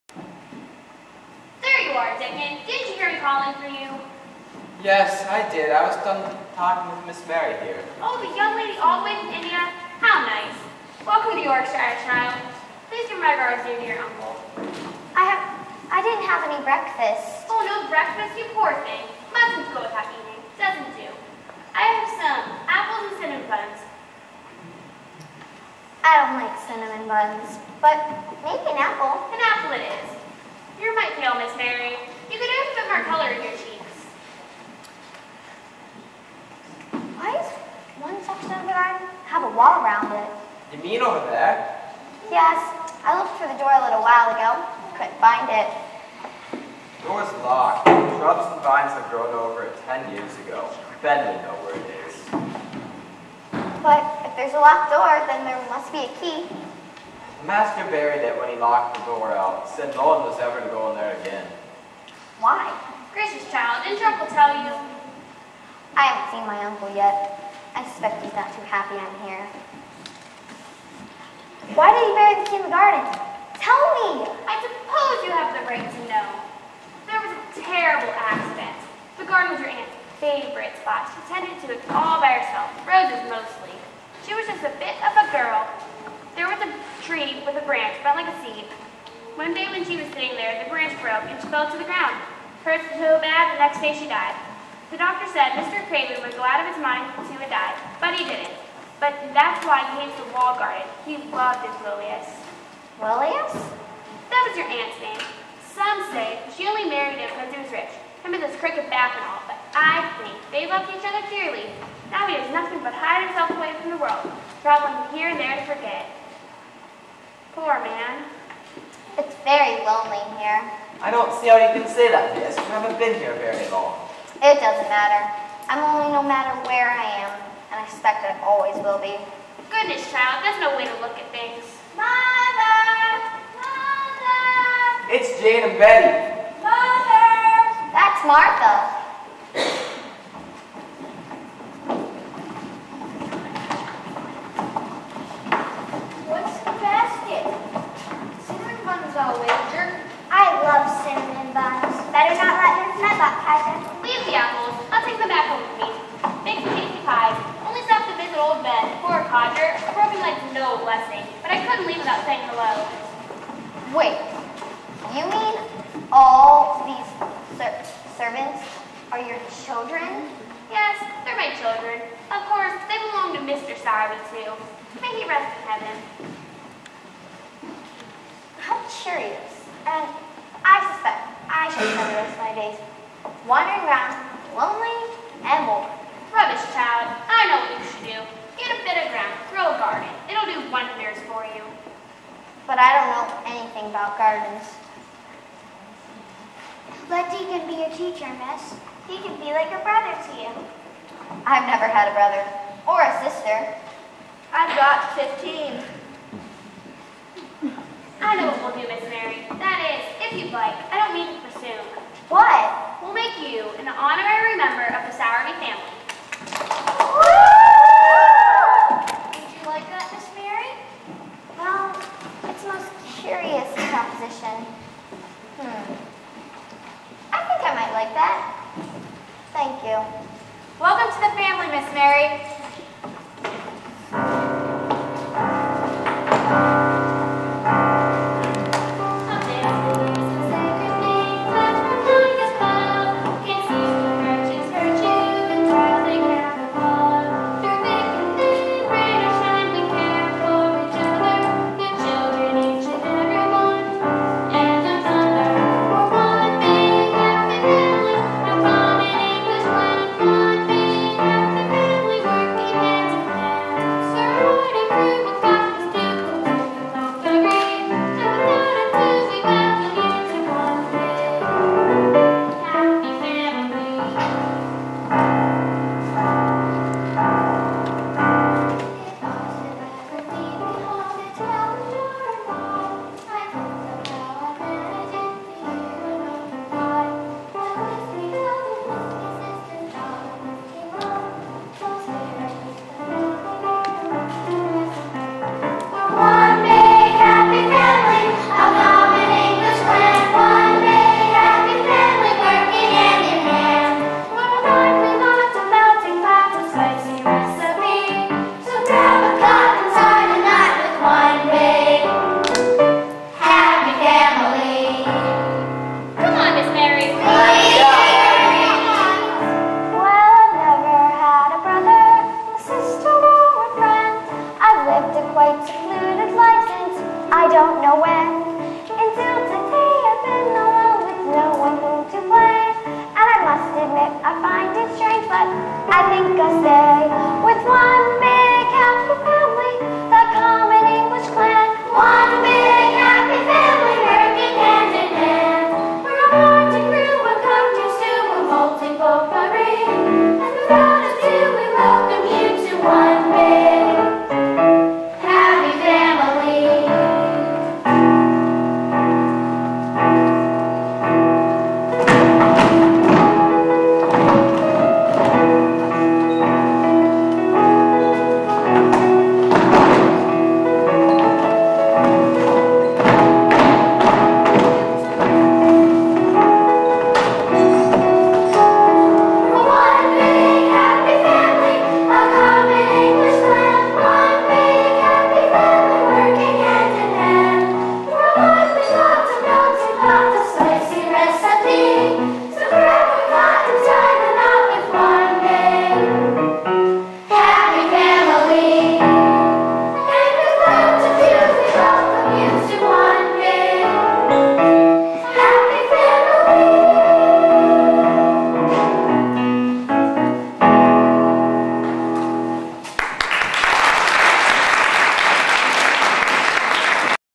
The Secret Garden
A scene from The Secret Garden as performed by the cast of Drama Days with the Louisville Community Theater in Louisville, Ohio.